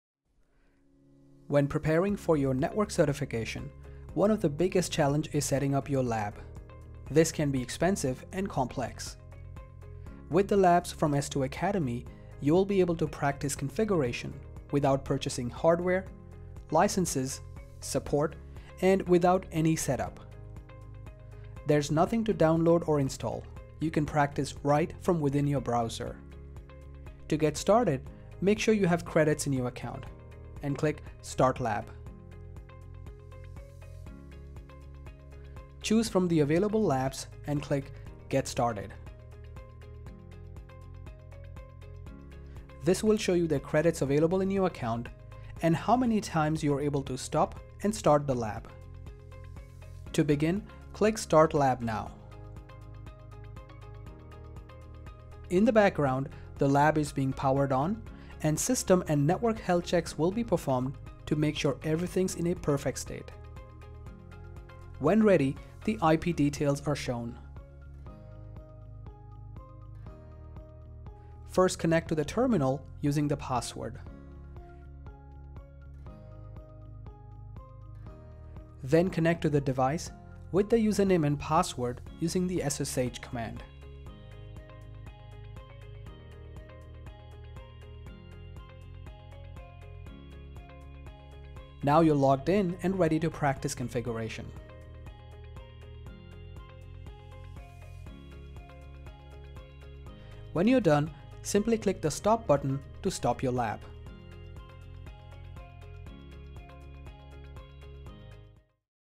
S2 Academy - Online Juniper Lab
Online, virtual Juniper SRX Lab from S2 Academy:
- fully online labs: no hardware, licenses or support to purchase
- connect right away: no need to reserve lab time
- nothing to install: connect directly from your browser
- free technical support: 24x7 support when you need it
- try risk-free: includes money-back guarantee